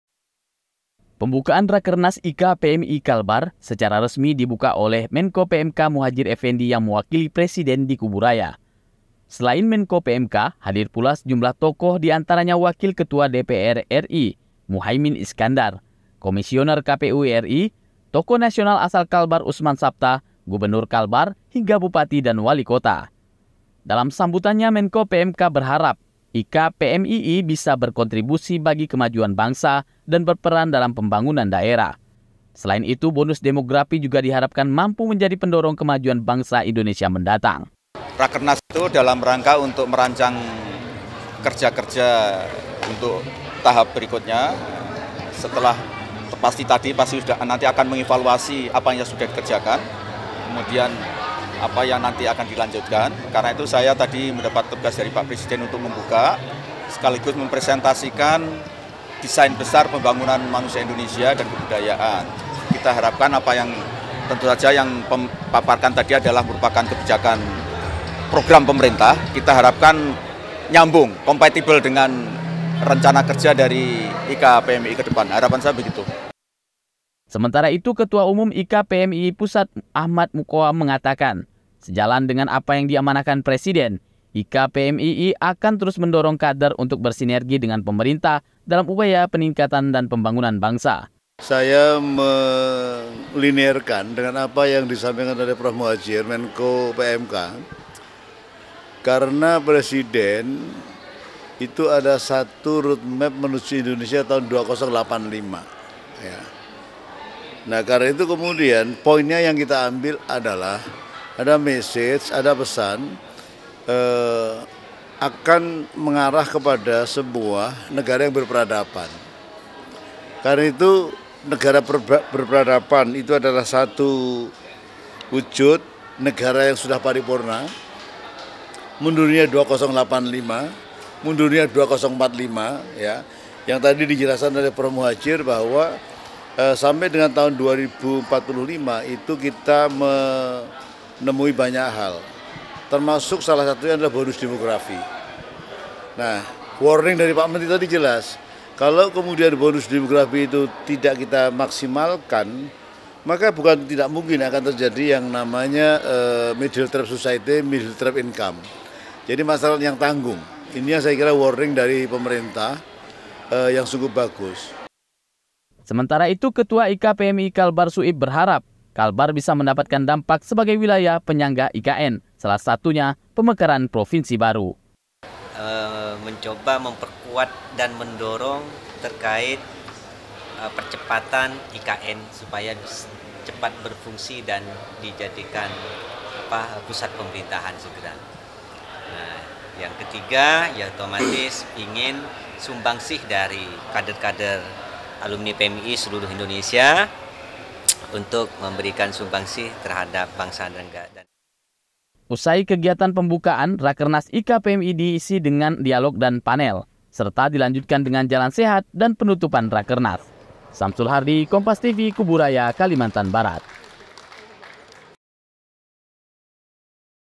Buka Rakernas IKA PMII, Menko PMK Dorong Kontribusi Bangun Daerah
KUBU RAYA, KOMPAS.TV - Pembukaan Rakernas IKA PMII Kalbar, secara resmi dibuka oleh Menko PMK Muhadjir Effendy yang mewakili presiden, di Kubu Raya.

Selain Menko PMK, hadir pula sejumlah tokoh, diantaranya Wakil Ketua DPR RI Muhaimin Iskandar, Komisioner KPU RI, tokoh nasional asal Kalbar Oesman Sapta, Gubernur Kalbar, hingga bupati dan wali kota.

Dalam sambutannya Menko PMK berharap, IKA PMII bisa berkontribusi bagi kemajuan bangsa, dan berperan dalam pembangunan daerah.

Selain itu, bonus demografi juga diharapkan mampu menjadi pendorong kemajuan bangsa Indonesia mendatang.

Sementara itu Ketua Umum IKA PMII Pusat Ahmad Muqowam mengatakan, sejalan dengan apa yang diamanahkan presiden, IKA PMII akan terus mendorong kader untuk bersinergi dengan pemerintah, dalam upaya peningkatan dan pembangunan bangsa.

Sementara itu Ketua IKA PMII Kalbar Suib berharap, Kalbar bisa mendapat dampak sebagai wilayah penyangga IKN, salah satunya pemekaran provinsi baru.

Usai kegiatan pembukaan, Rakernas IKA PMII diisi dengan dialog dan panel, serta dilanjutkan dengan jalan sehat, dan penutupan rakernas.